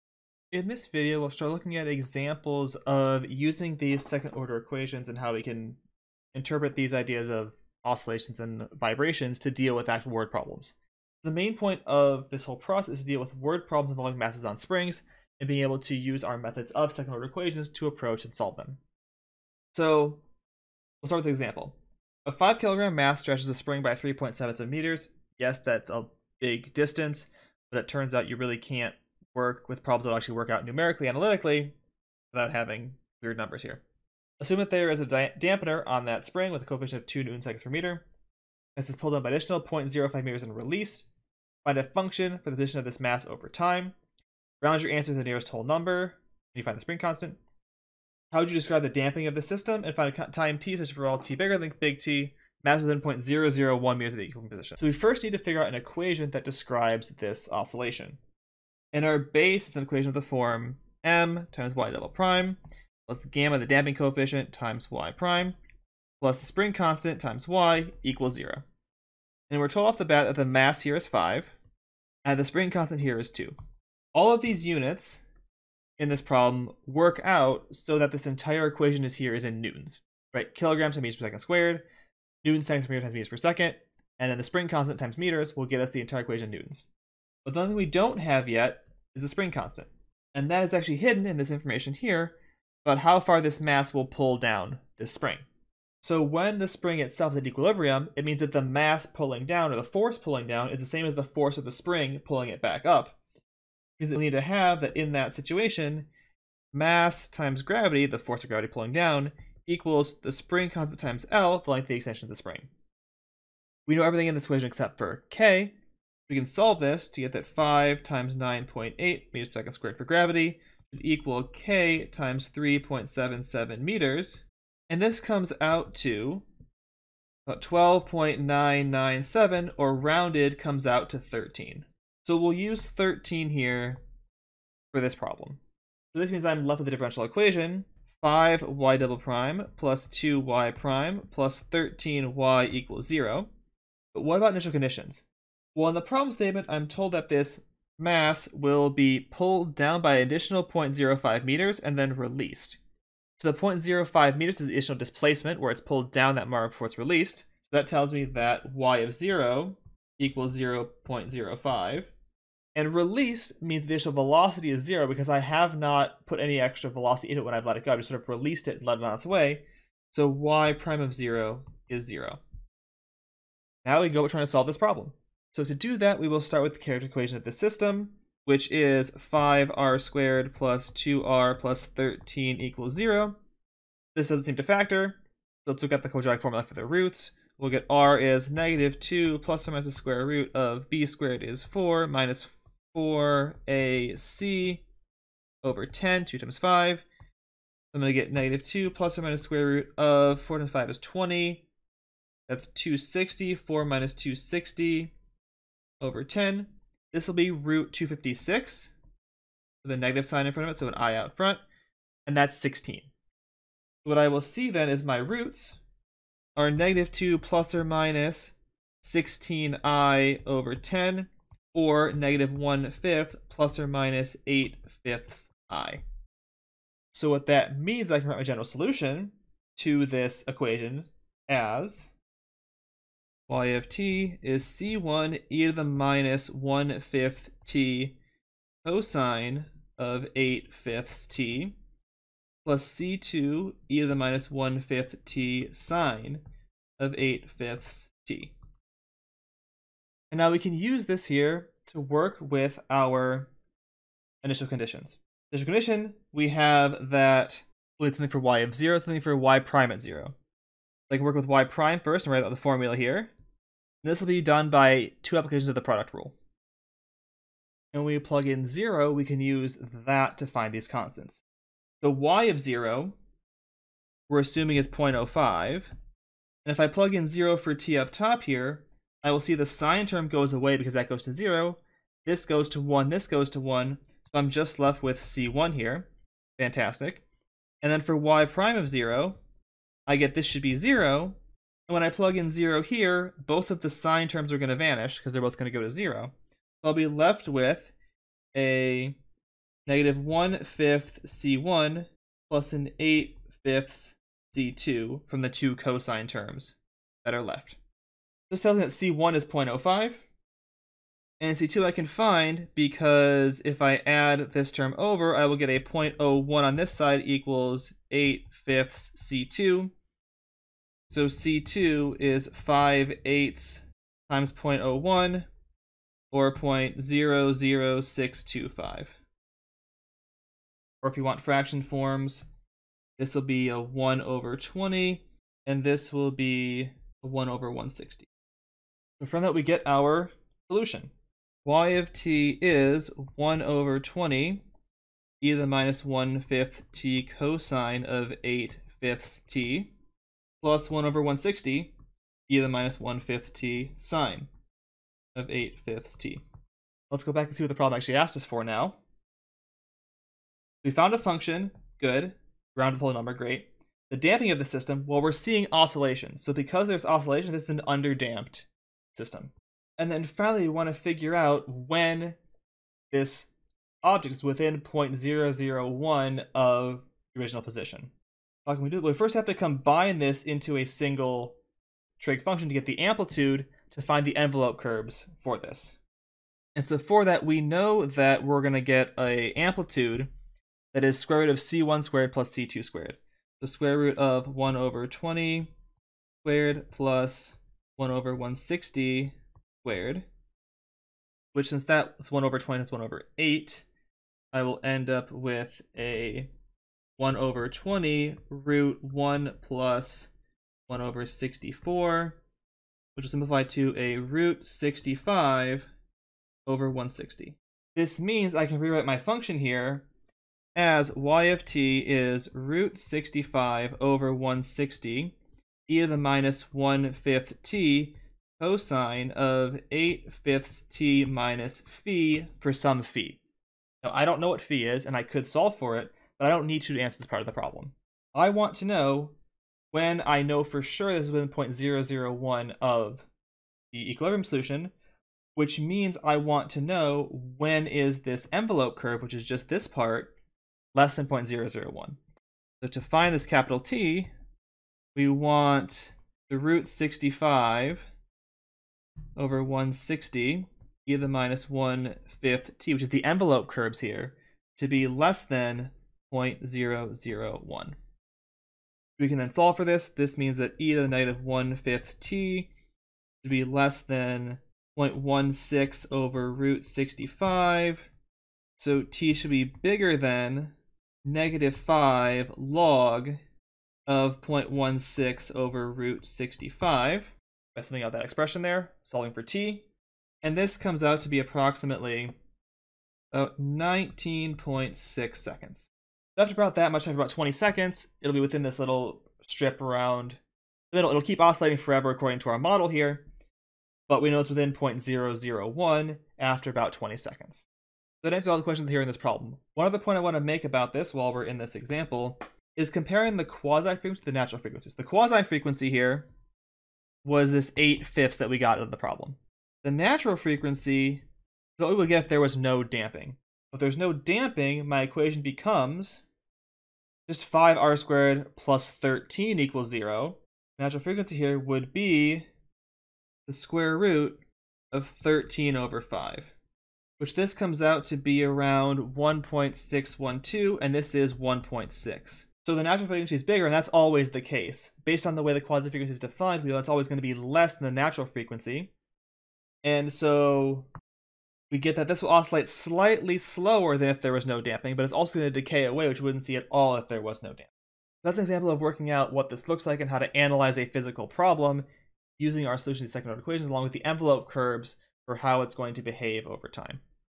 Differential Equations - Mechanical and Electrical Vibrations - Example 1
Video showing an example of analyzing a physical problem with a mass on a spring using methods of second order equations. Use of the envelope curves to determine when the solution is within certain bounds is also covered.